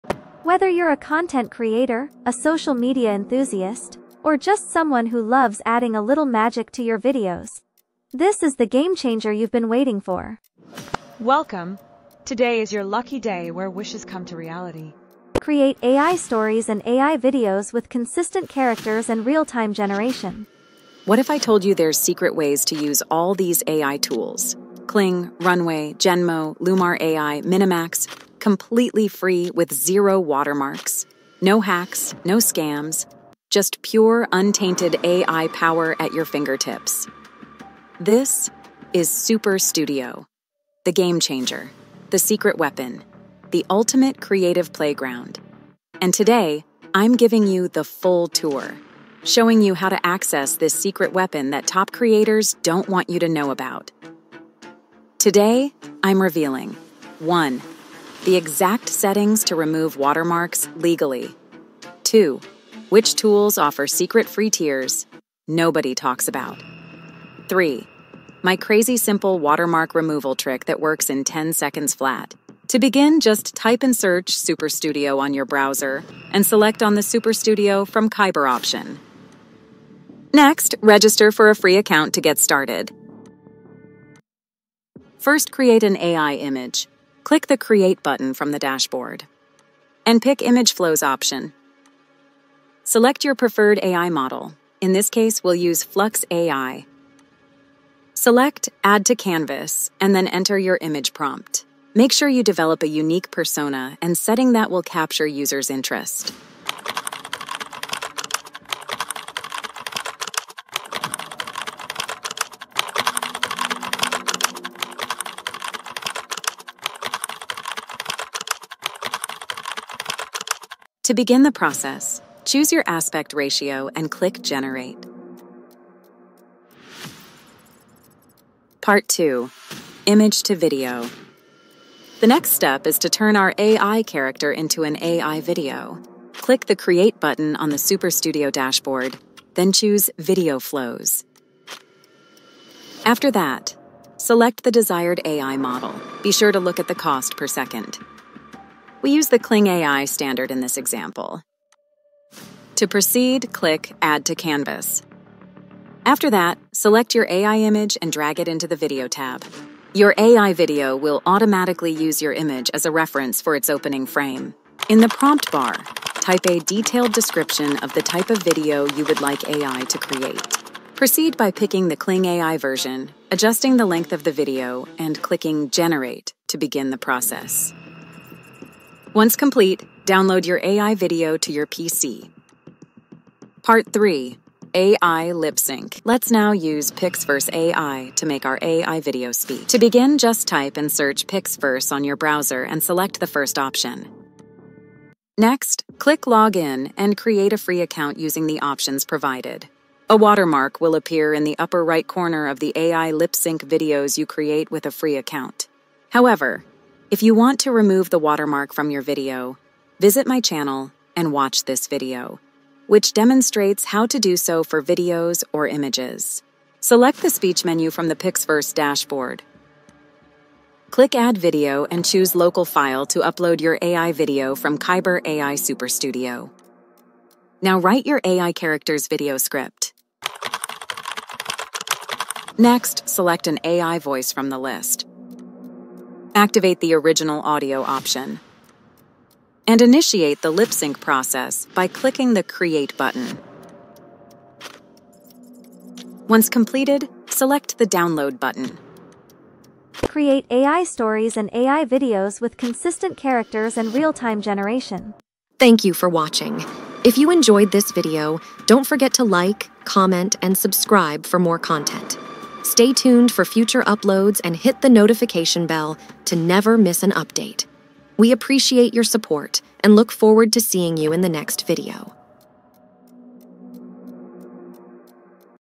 Superstudio AI Video Generator No Watermark + Lipsync Kaiber Pixverse
"What if I told you there's secret ways to use ALL these AI tools - Kling, Runway, Genmo, Lumar AI, Minimax - COMPLETELY FREE... with ZERO watermarks? No hacks. No scams. Just pure, untainted AI power at your fingertips."
This… is SUPER STUDIO. The game-changer. The secret weapon. The ultimate creative playground.And today, I'm giving you the FULL tour - showing you how to access this secret weapon that top creators don't want you to know about!"
"Today I'm revealing:
1️⃣ The EXACT settings to remove watermarks legally
2️⃣ Which tools offer secret free tiers NOBODY talks about
3️⃣ My crazy simple watermark-removal trick that works in 10 seconds flat
...and why some of these tools might BAN you if you do this wrong!"
TABLE OF CONTENTS:
00:00 INTRO  Superstudio AI Video Generator No Watermark + Lipsync Kaiber Pixverse
01:16  Superstudio from Kaiber
02:14 image to video
03:06 AI Lip sync
03:08 Pixverse
04:16 Outro